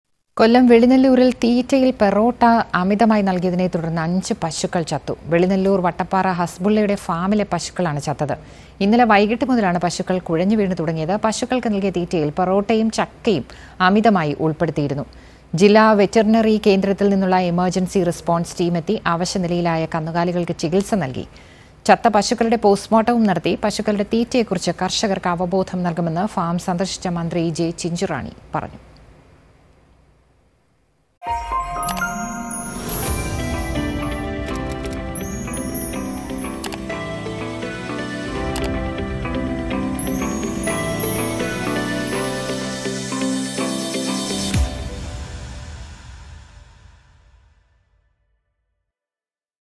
തീറ്റയിൽ അമിതമായി പൊറോട്ട നൽകിയതിനെ തുടർന്ന് അഞ്ച് പശുക്കൾ ചത്തു
തീറ്റയിൽ അമിതമായി പൊറോട്ട  നൽകി; കൊല്ലത്ത് അഞ്ച് പശുക്കൾ ചത്തു